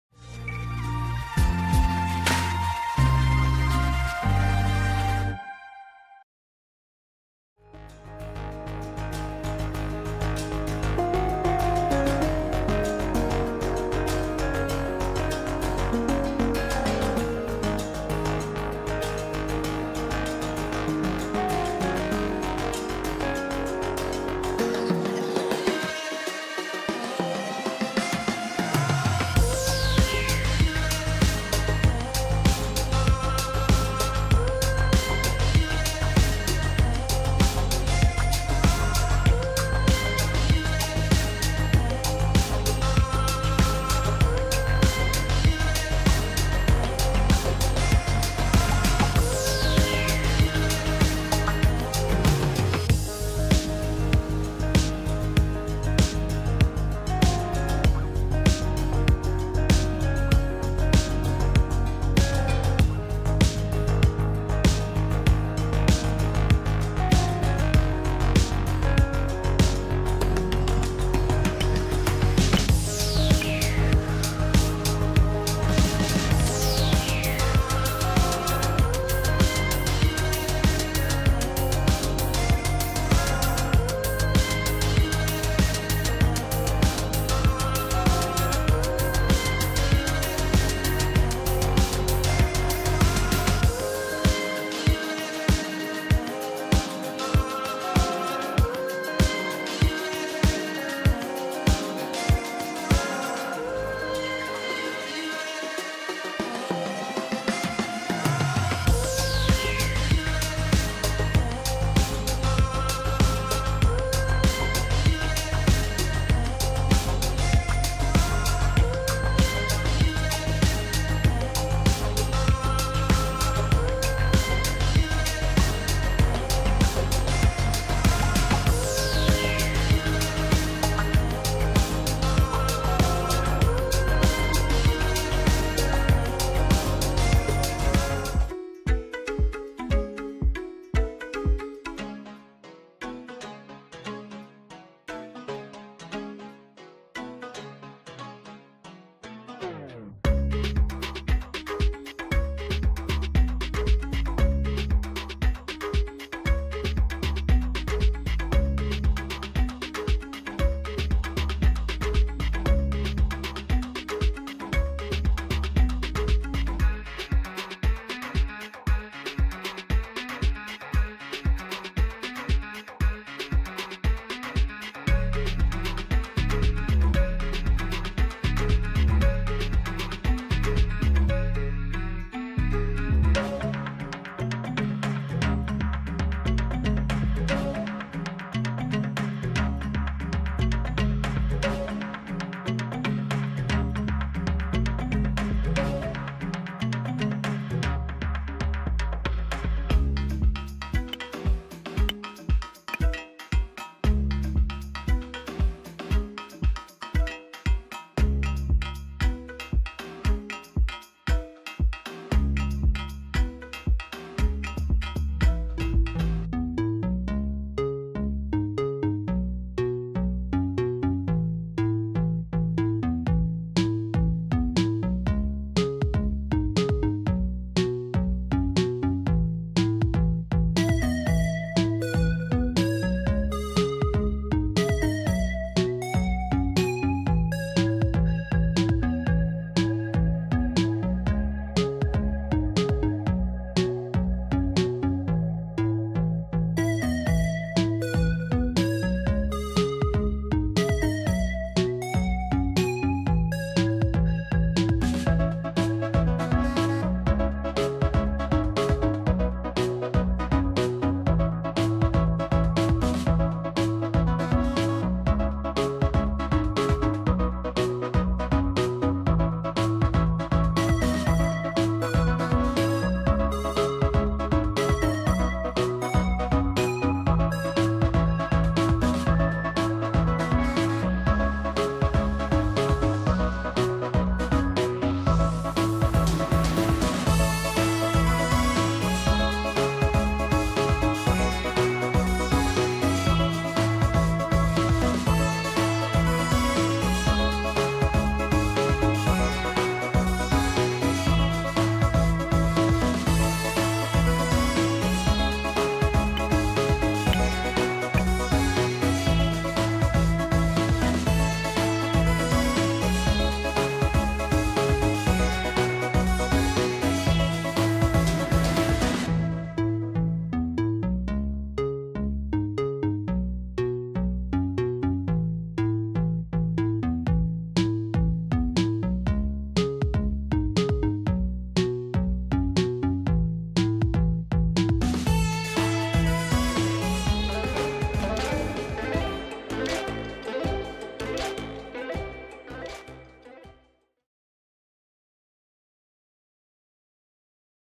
Nail Art Designs 2020 | Marble Nails Art
🔴 Products used in this video (without brand names):
nail polish/gel, striping tape, silicone stamper, soft sponge, skin defender, glossy/matte top coat, caviar beads, glue, rhinestones (UV lamp, dotting tools, brushes, tweezers, wax picker pen...)